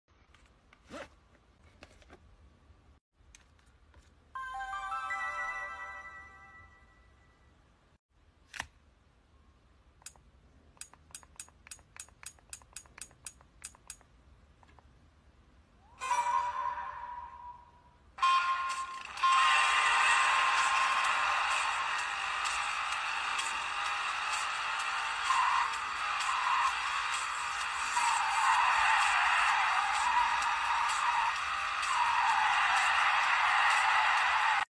Wer erinnert sich noch an die PSP Go? 👴🧓
Die PSPGo ist vielleicht die unterschätzteste Playstation Konsole aller Zeiten.Aber wer eine hatte weiß: Die PSP Go war richtig nice!
Kompakter Gaming Spaß mit absoluten Highlight-Titeln! Wir wollen die PSP Go und auch die normale PSP und PSVita zumindest nicht in unserer Konsolenbibliothek missen!

Bei uns hört der PlayStation-Content aber noch nicht auf! Es gibt viel zu erleben, also guckt doch auch mal auf den anderen Kanälen vorbei.